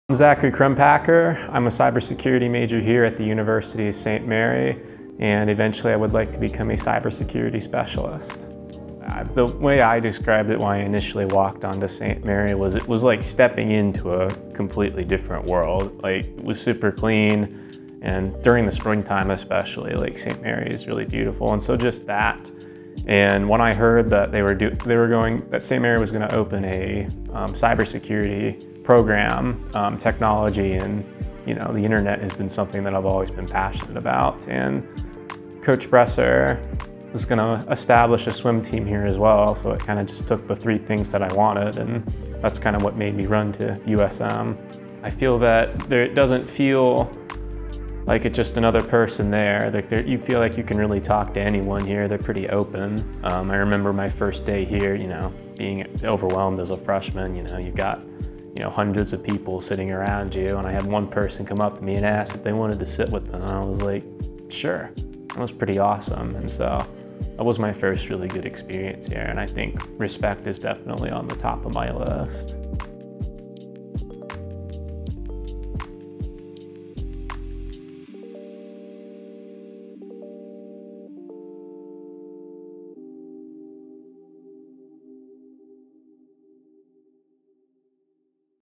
Zachary Crumpacker: The Student-Athlete Experience
University of Saint Mary cybersecurity student Zachary Crumpacker found the perfect fit at USM thanks to a combination of rigorous academic programs and the family atmosphere within Spire Athletics.